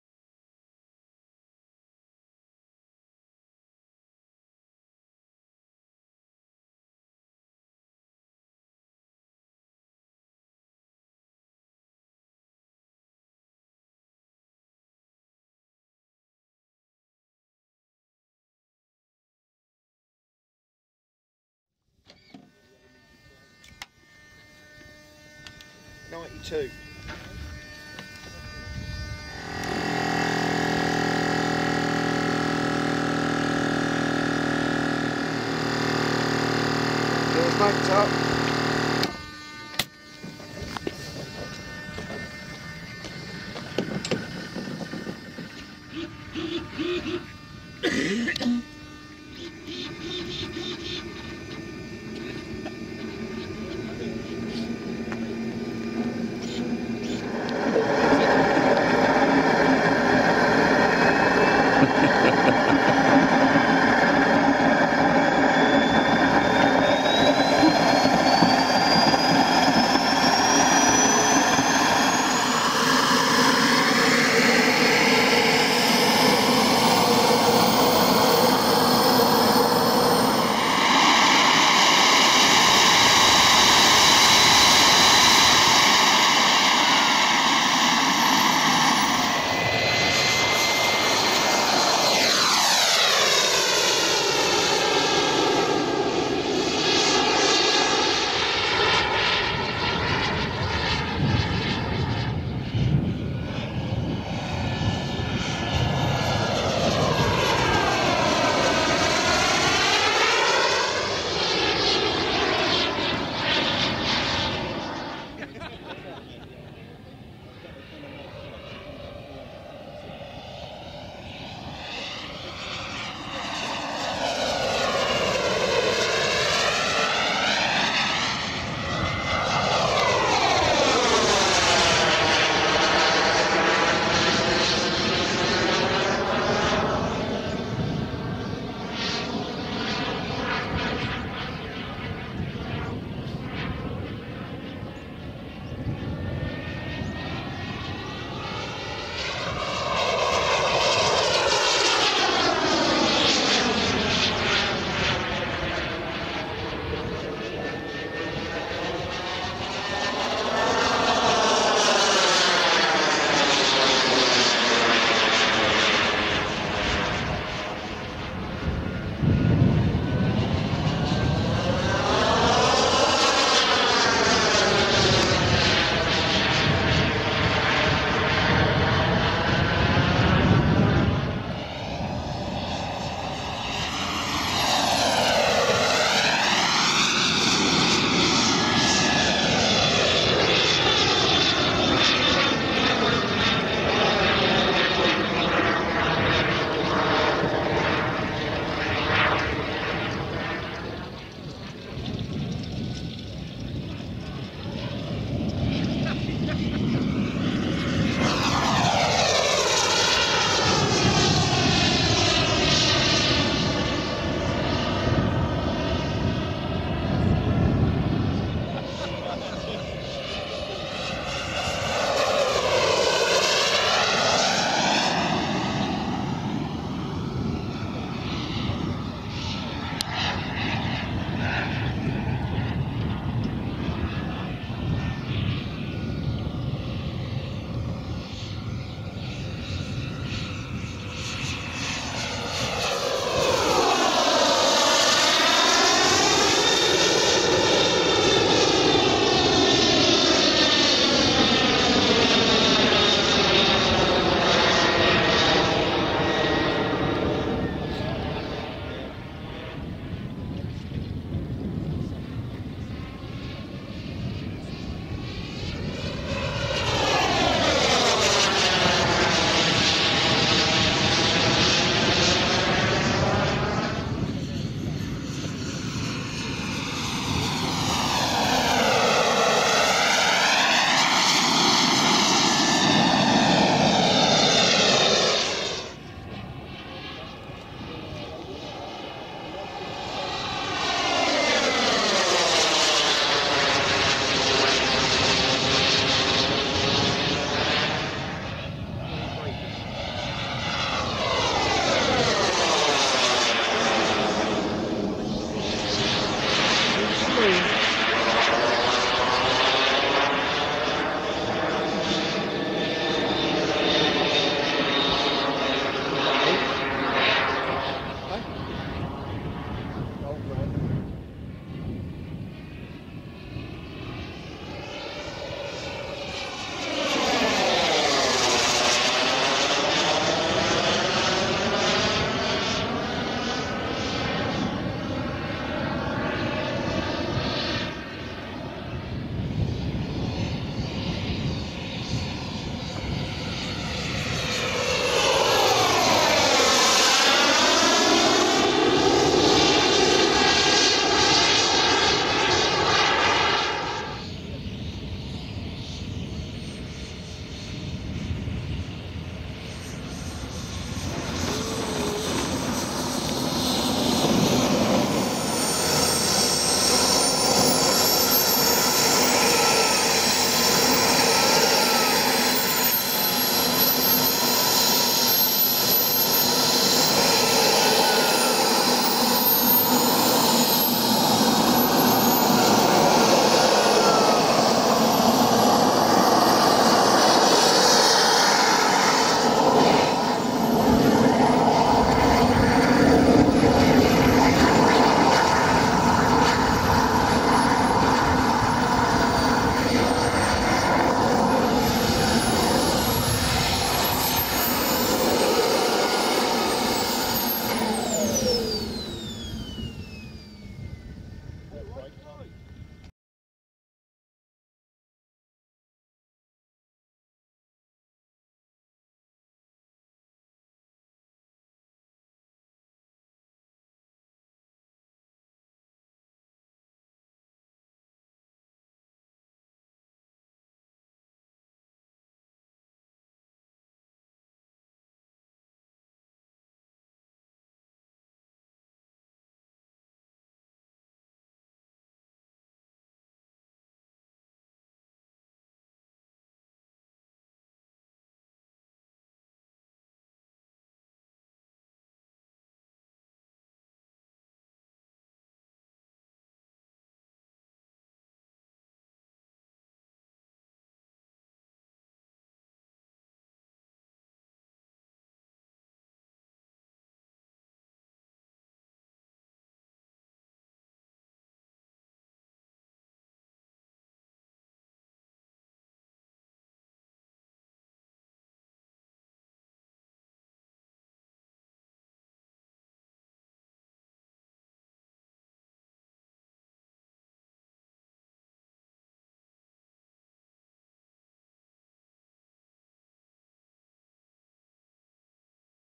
TOMAHAWK L-39 ALBATROS JET TRAINER RC GIANT SCALE - TIBENHAM TRIBUTE FLY-IN - 2016
MARK HINTON TOOK THE CONTROLS OF THIS AWESOME GIANT SCALE DELFIN - BUILT AND OWNED BY PETER LANCASTER AT THIS BATTLE OF BRITAIN TRIBUTE FLY - IN AT RAF TIBENHAM AN EX AMERICAN BOMBER BASE IN NORFOLK UK,
THIS FLY - IN WAS ORGANIZED BY PHIL GOFF - MANY THANKS PHILL IT WAS A GREAT DAY !

REAL AIRCRAFT INFO -

The Aero L-39 Albatros is a high-performance jet trainer aircraft developed in Czechoslovakia by Aero Vodochody. It was designed during the 1960s as a replacement for the Aero L-29 Delfín as a principal training aircraft. The L-39 Albatros has the distinction of being the first of the second-generation jet trainers to be produced, as well as being the first trainer aircraft to be equipped with a turbofan powerplant. The type was widely exported to a wide range of international operators as a military trainer.
The L-39 Albatros later served as the basis for the updated L-59 Super Albatros, as well as the L-139 (prototype L-39 with Garrett TFE731 engine). A further development of the design, designated as the L-159 ALCA, entered production in 1997. To date, more than 2,800 L-39s have served with over 30 air forces around the world. The Albatros is the most widely used jet trainer in the world; in addition to performing basic and advanced pilot training, it has also flown combat missions in a light-attack role. Atypically, it has never received a NATO reporting name.
At the Farnborough Airshow in July 2014, Aero Vodochody announced the launch of the L-39NG, an upgraded and modernised version of the L-39.

FILMED AT A BATTLE OF BRITAIN MEMORIAL FLY- IN AT THE SECOND WORLD WAR LIBERATOR BOMBER AIRBASE OF TIBENHAM IN NORFOLK ON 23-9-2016,